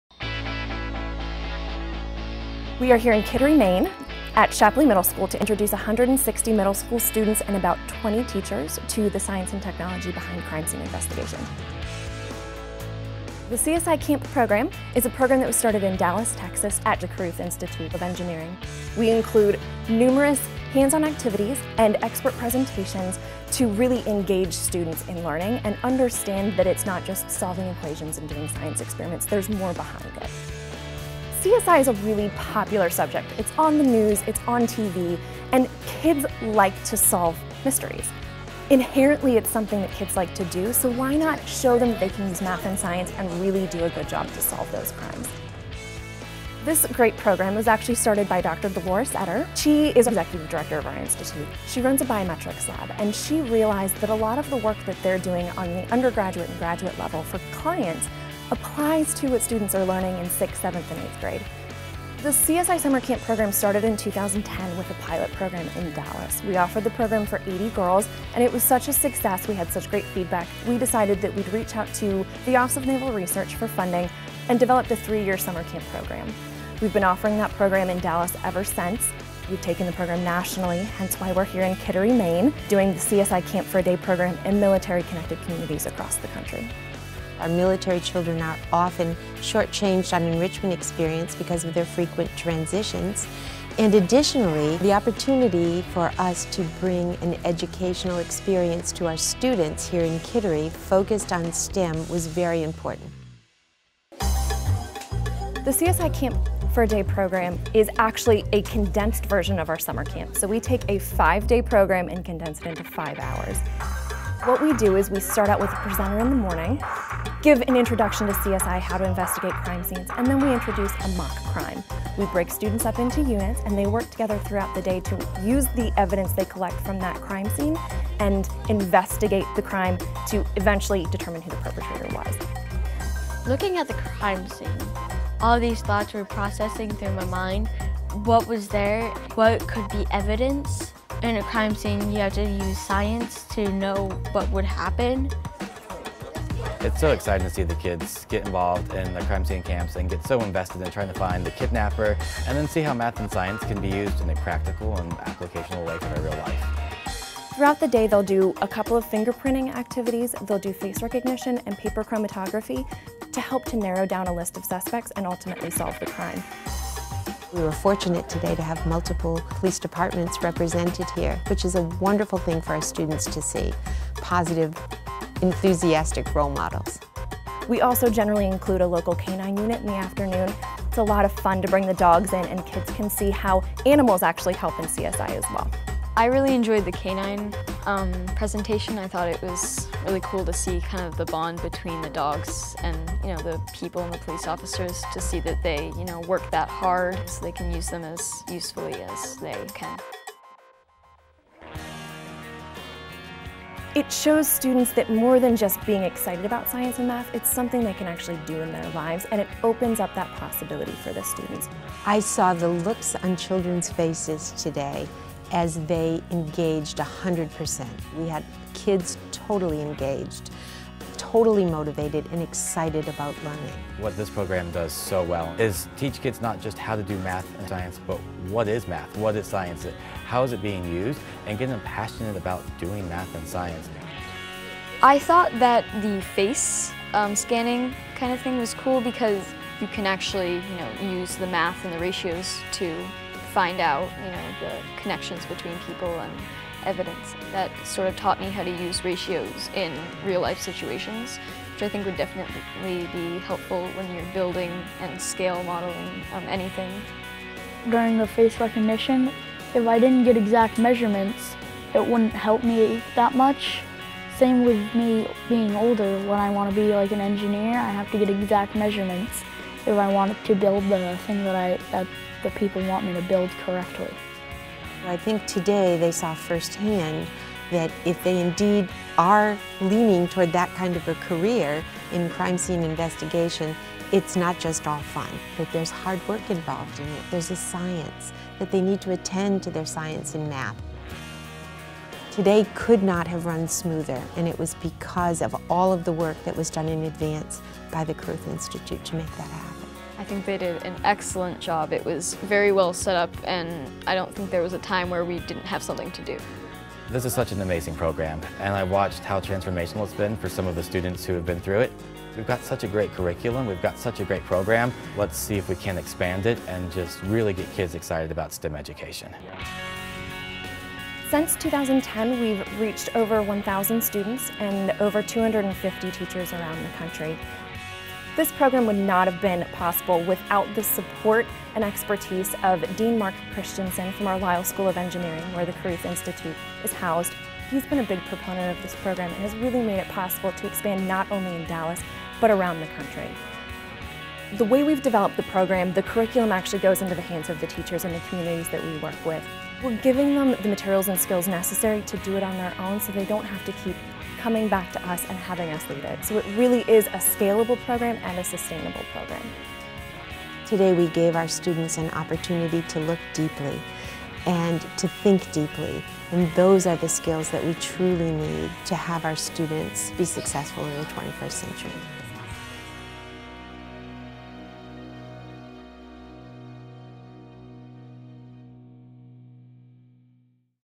CSI Summer Camp Program - Caruth Institute for Engineering Education, Southern Methodist University
In order to get kids excited about the sciences, the Caruth Institute for Engineering Education -- part of Lyle School of Engineering at Southern Methodist University, TX -- runs a summer camp program for kids with a healthy interest in solving crimes. With the collaboration of the military, local police officers, and schools across the country, kids solve a "crime" by working together, learning the tricks of the trade and getting a whole new insight into what they can do with science.